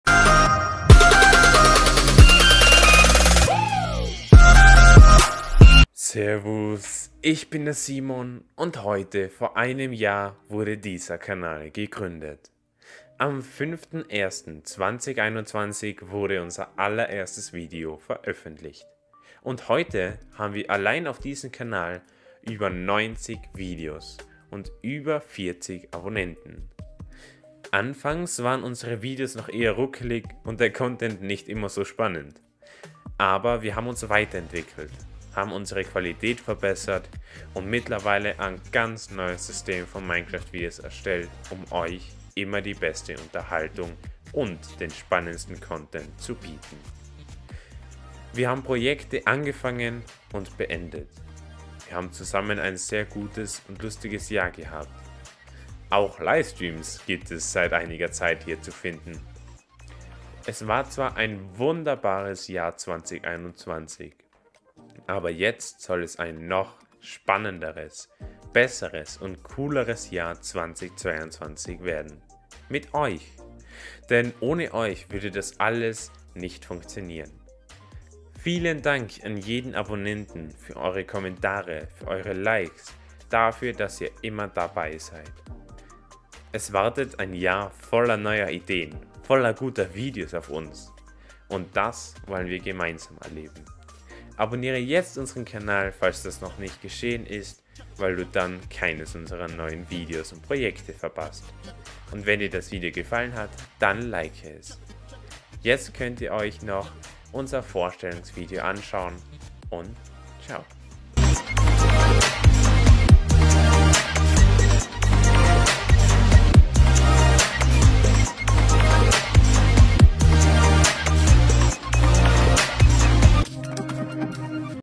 1 Jahr Klotzi LP
Ein Jahr Klotzi LP, kaum zu fassen. So viel erlebt und so viel gelernt. Darauf sind wir sehr stolz und freuen uns auch wirklich unser Hobby so ausführen zu können. Vielen vielen Dank, an jeden von euch dort draußen, da ihr immer für uns da seit.